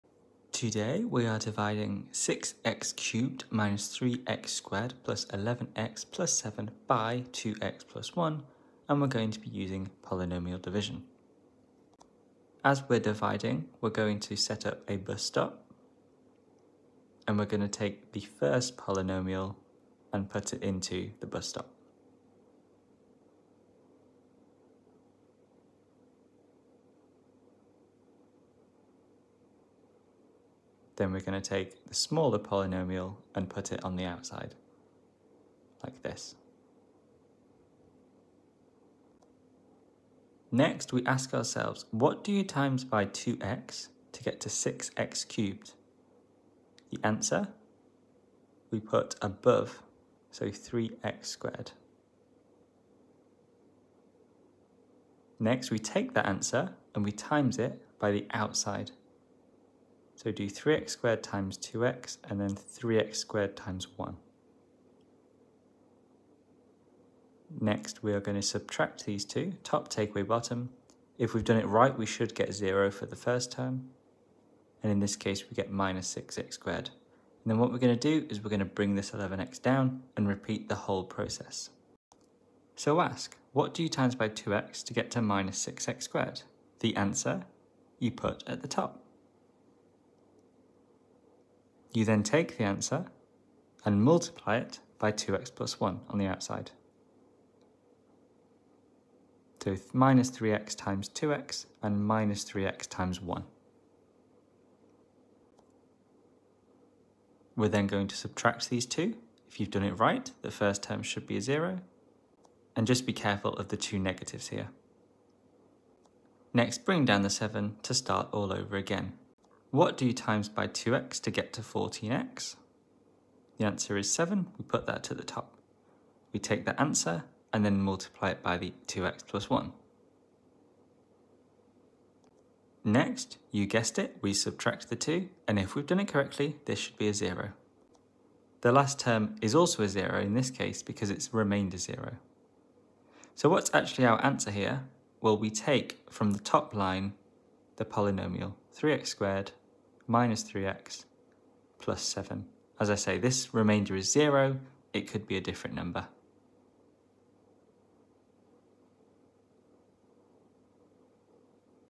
AS August Day 23 - Dividing Polynomials
If you're struggling with AS maths, then come check out this video. On the twenty-third day of AS August, we're having a look at dividing polynomials, a key topic in the syllabus. Could you answer the question or did you need a hint?